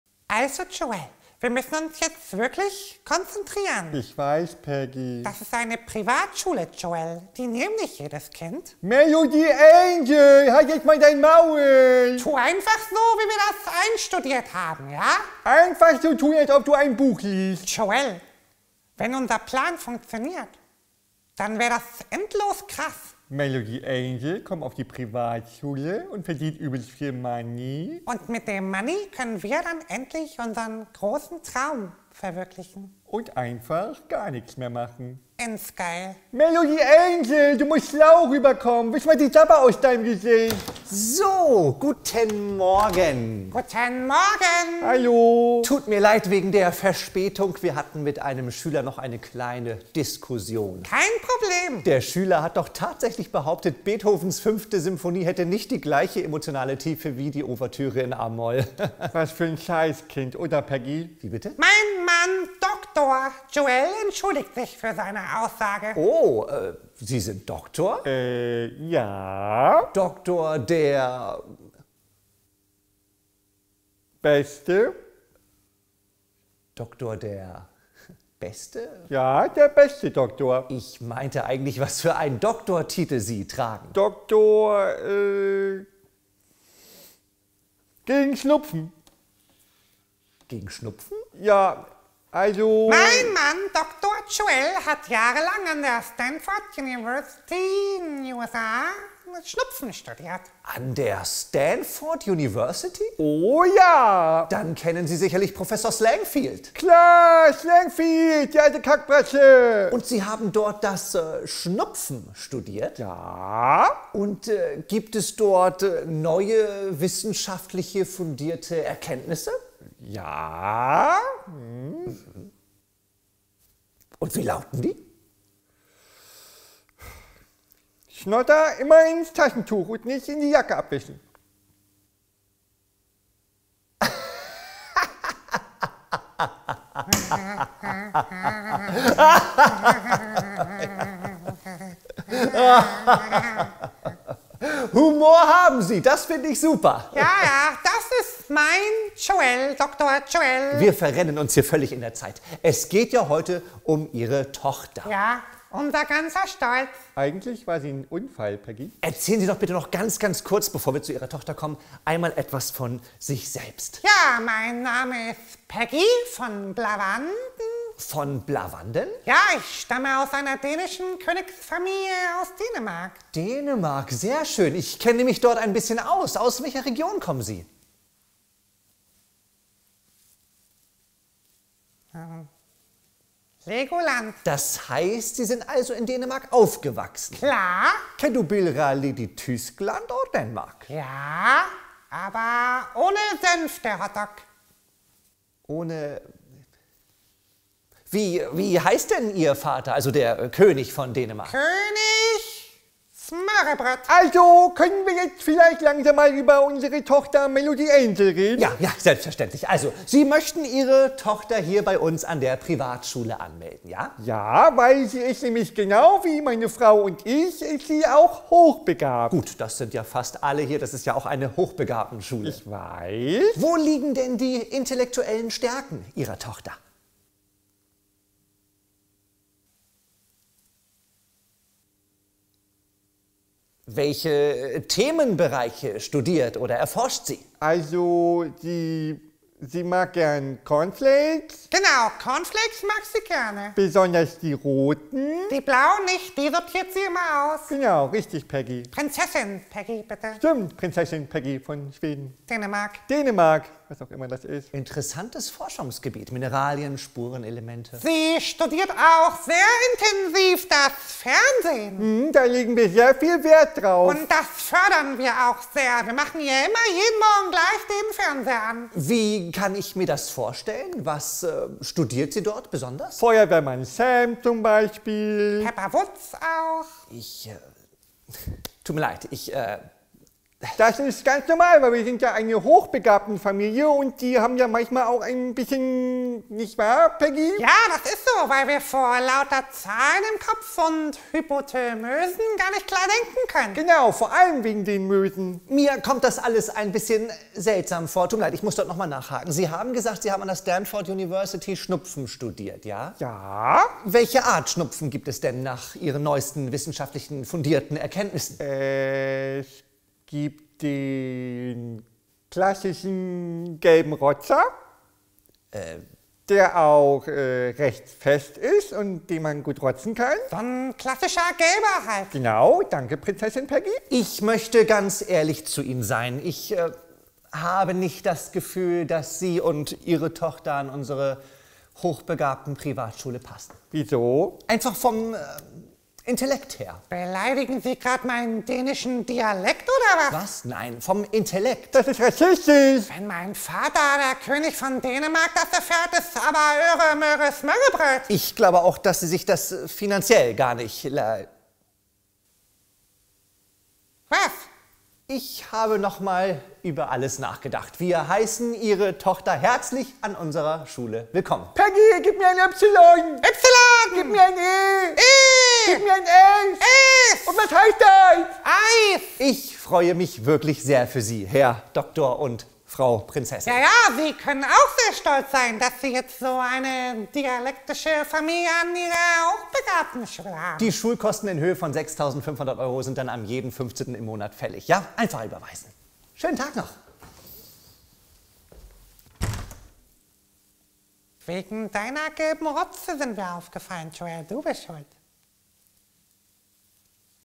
Peggy & Joel versuchen ihre Tochter an einer Hochbegabten-Schule anzumelden👩‍🎓 | Freshtorge
Die jungen Eltern haben einen Plan, wie sie in Zukunft weiter faul auf ihrem Sofa liegen können. Dazu müssen sie es nur schaffen, ihre Tochter Melodie Angel an einem Hochbegabten Gymnasium anzumelden!